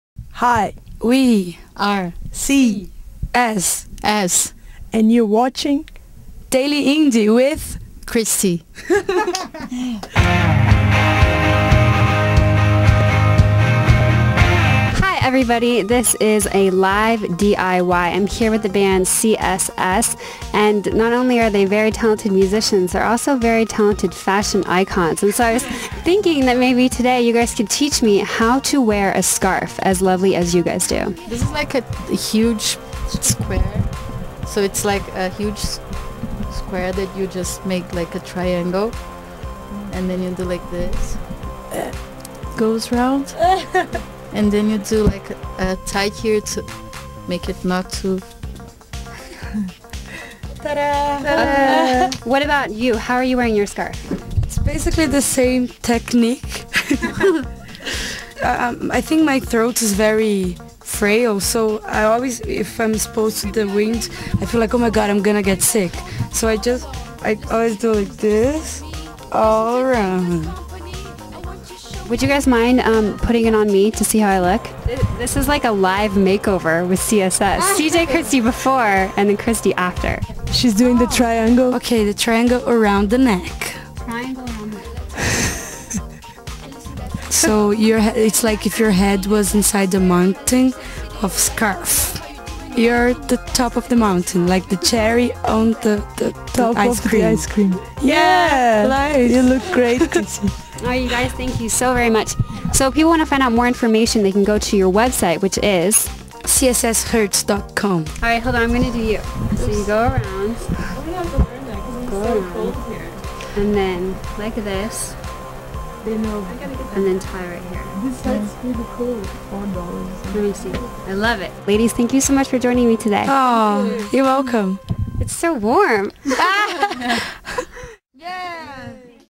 CSS Give Christy A Lesson On Fashion
The band CSS teaches Christy how to tie a scarf in the most fashionable of ways.